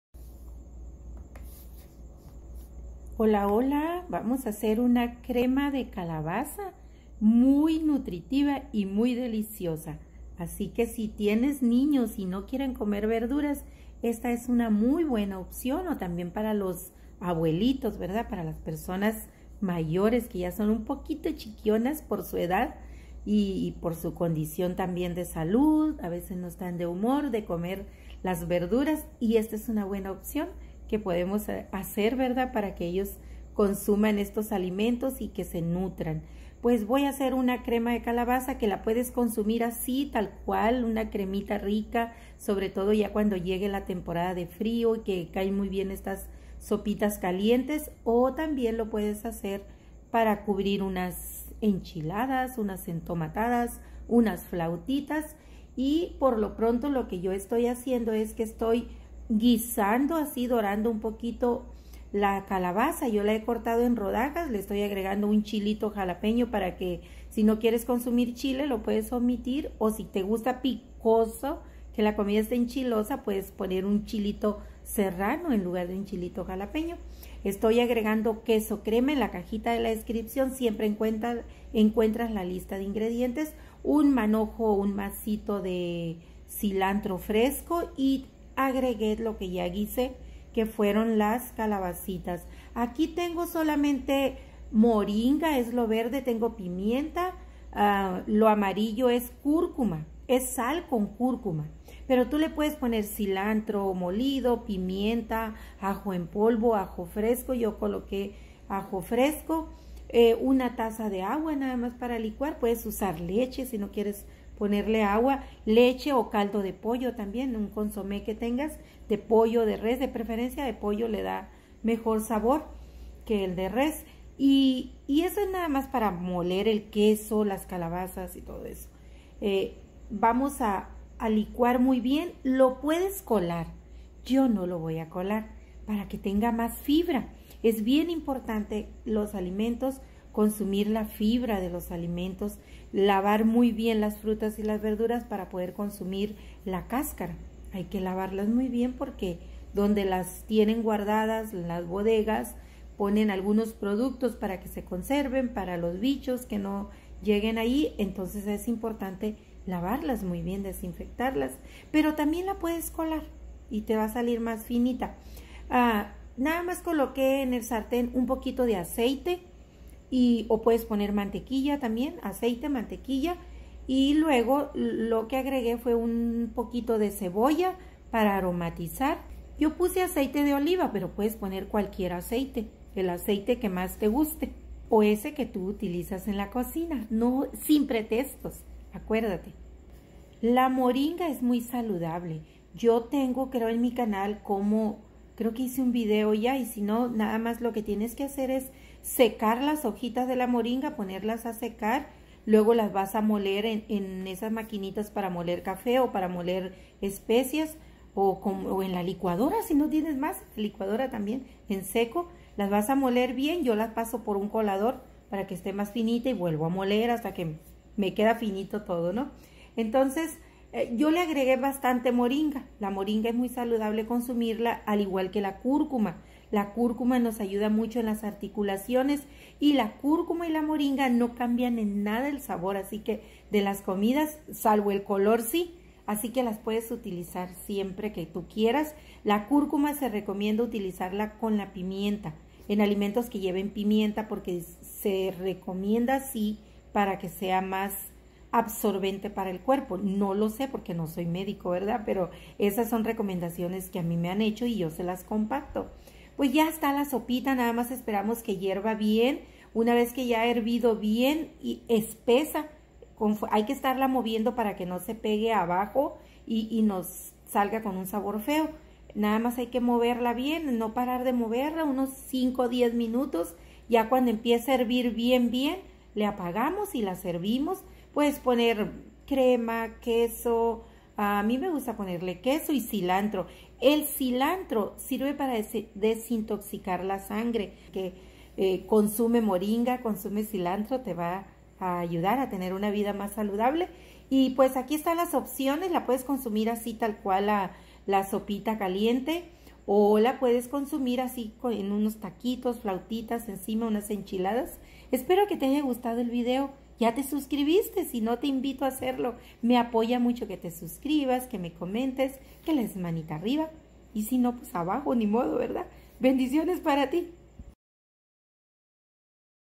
Crema de Calabacín con Moringa! - SinPretextos.
Aquí un video de como se preparé esta sabrosa crema de calabaza. Estos son los INGREDIENTES:
1 calabacín tierno mediano.
1/2 barra de queso crema.
1 chile jalapeño o serrano si prefieres que enchile.
1 puñado de ramas de cilantro fresco.
2 dientes de ajo.
1 ó 2 tazas de agua o caldo de pollo.
1 cucharadas de aceite y un trocito de mantequilla.
1 cucharada de cebolla finamente picada.
Condimentar con: pimienta, cúrcuma y moringa o al gusto.
                     GRACIAS POR VISITAR ESTE  BLOG Y VER EL VÍDEO
GRUPO DE FACEBOOK    "COCINA RICO SIN PRETEXTOS"
PÁGINA DE FACEBOOK  "COCINA RICO CON HUERTO EN CASA"